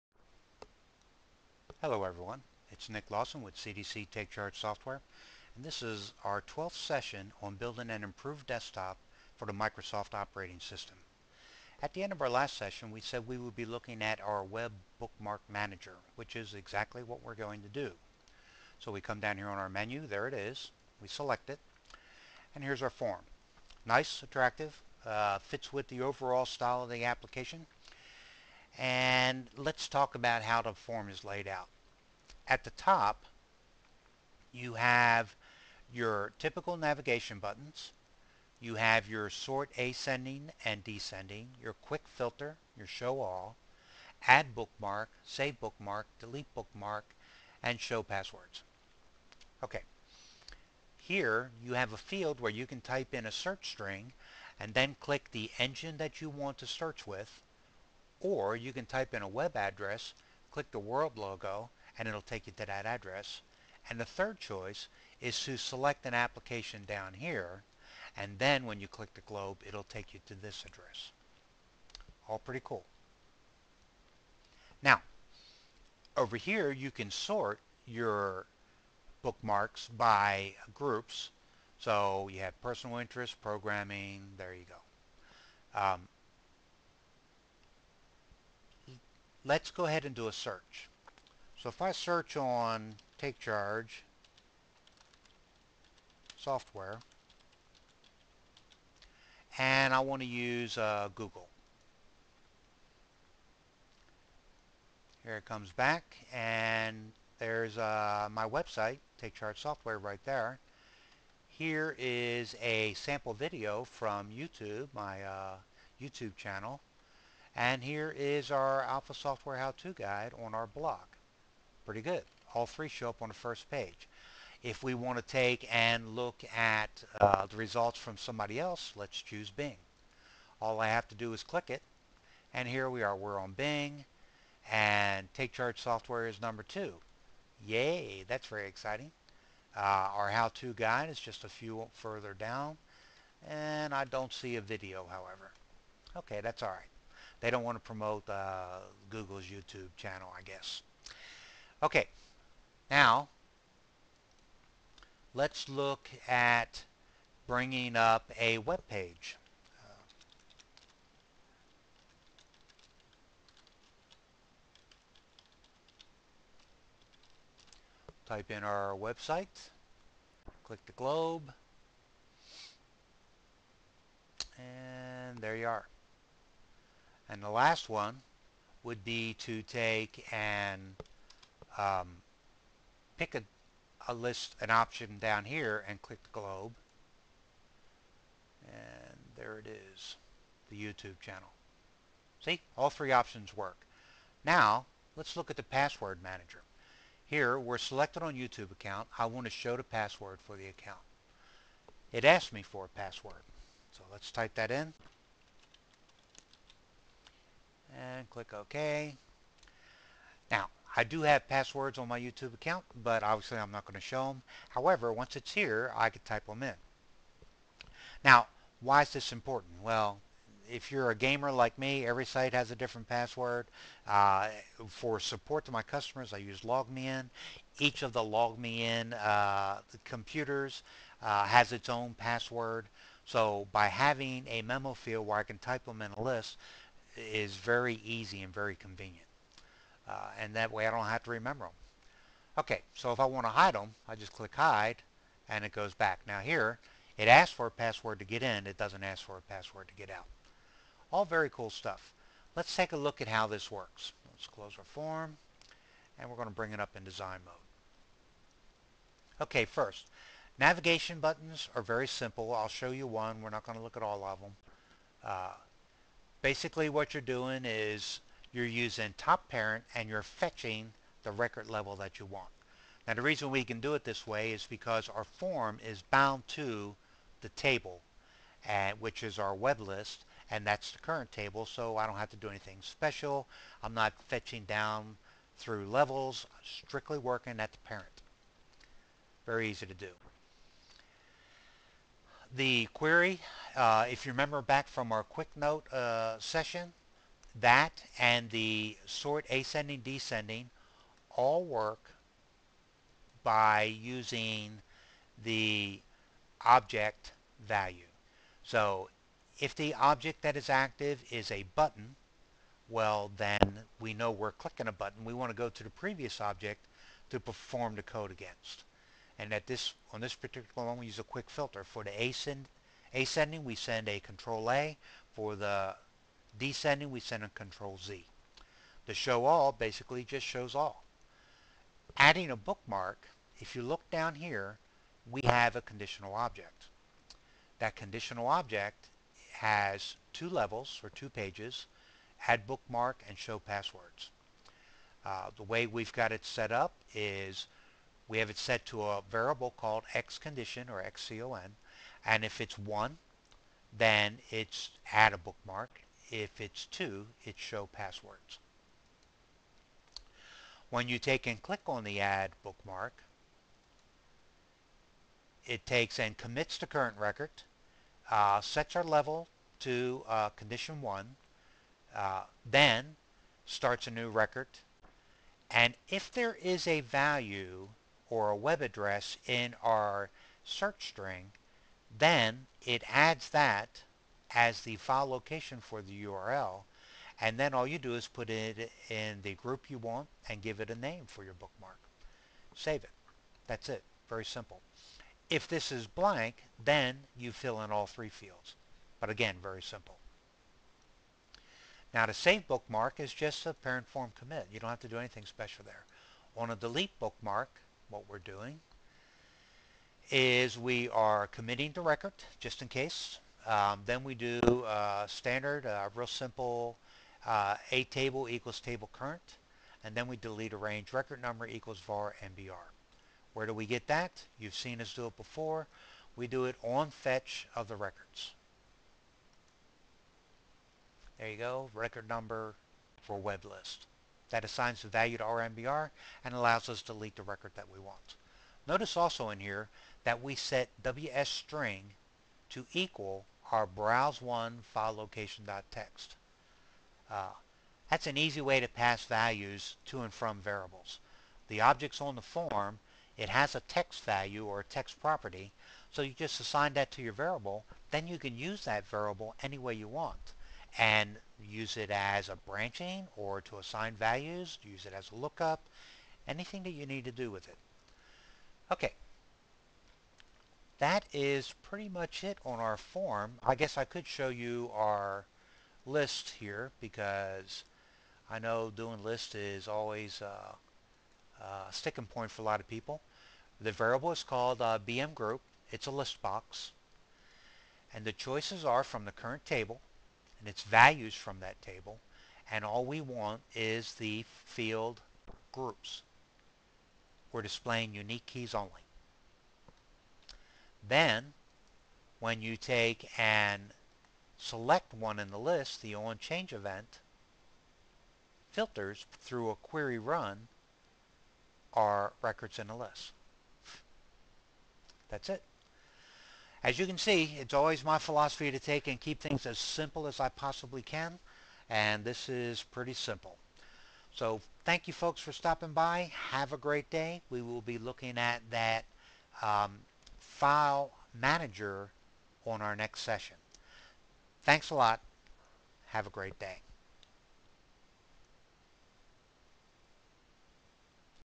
PC Desktop Bookmark Manager
Today's session looks at the Web Bookmark Manager we designed for our new desktop. Sit back relax and enjoy the show. If you want to see examples of the coding and design, go to our blog at